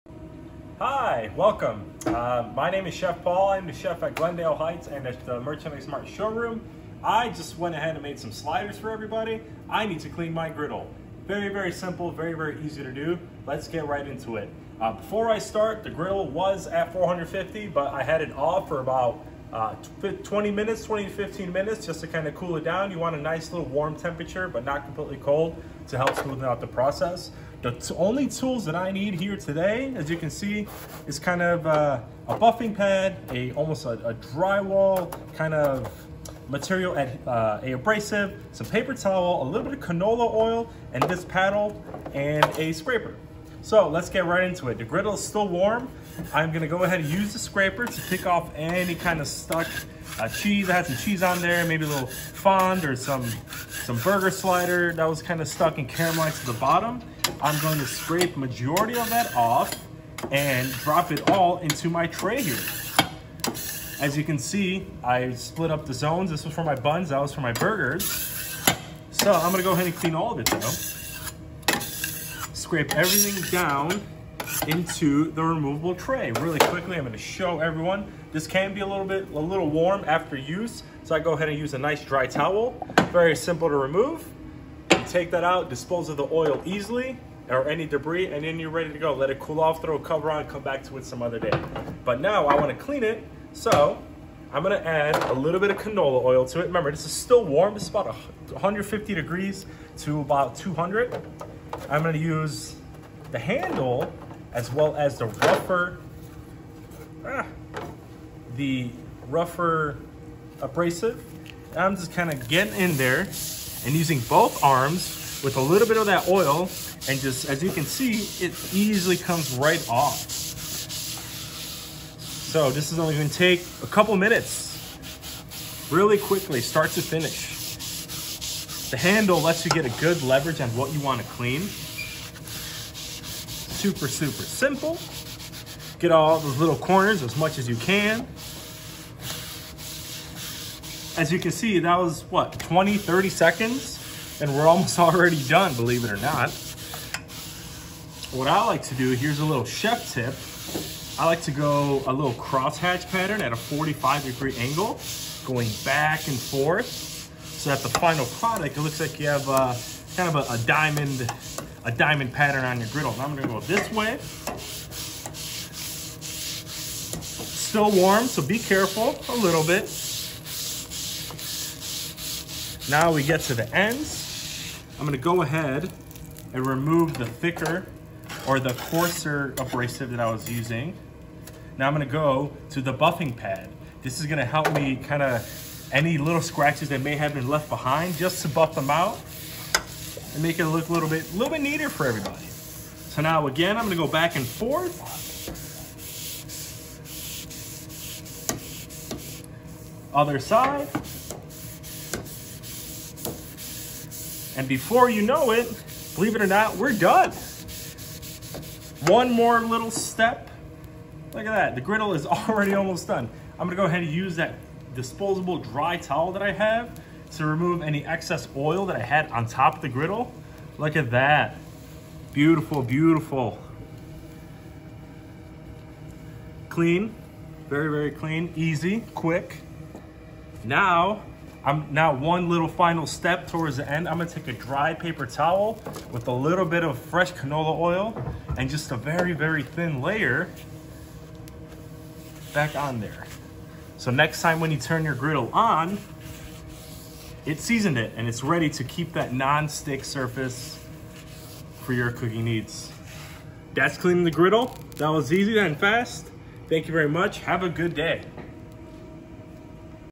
Cleaning a Wolf Griddle - how to
Cleaning a Wolf brand single or double griddle is easy.

When cooking is finished, turn the griddle down to a low temperature, use a bit of canola oil, then scrub with the scrub pads that come with most griddle cleaning kits. Push everything towards you into the drip tray, then dispose. When finished, spread a very thin layer of canola oil to prevent oxidation until next use - the griddle will smoke a bit when you turn it on next time.  When that smoke stops, all the previous oil will be burnt off and everything will be ready to go. Easy. Simple. Awesome.

Want more Sub-Zero, Wolf, and Cove cleaning tips? Check out the other videos in this playlist (recipes too):